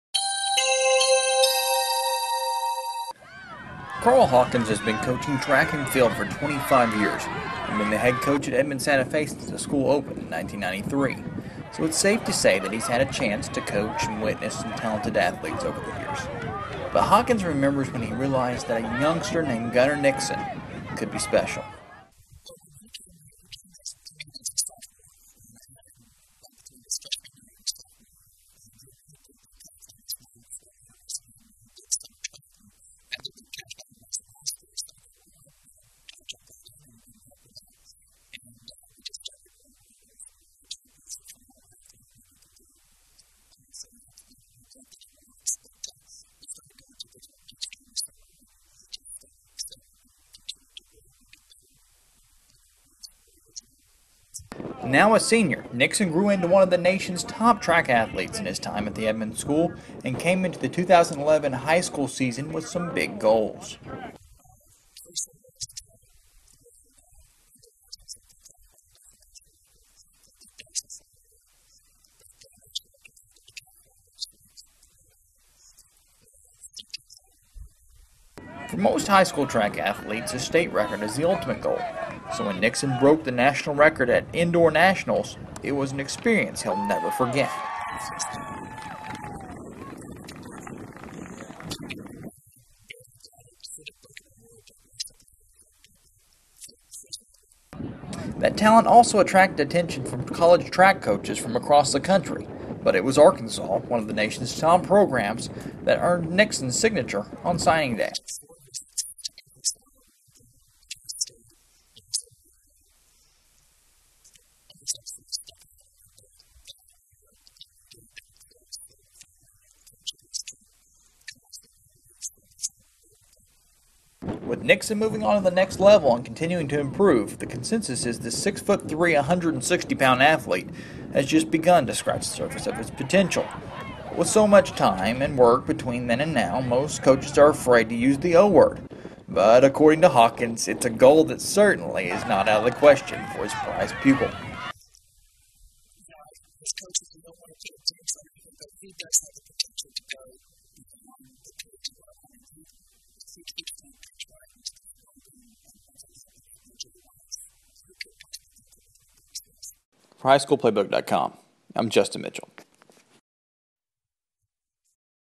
High School Profile: Edmond Santa Fe's Gunnar Nixon
High School Playbook's Justin Mitchell talks with University of Arkansas-bound track athlete Gunnar Nixon and his Edmond Santa Fe coach, Carl Hawkins.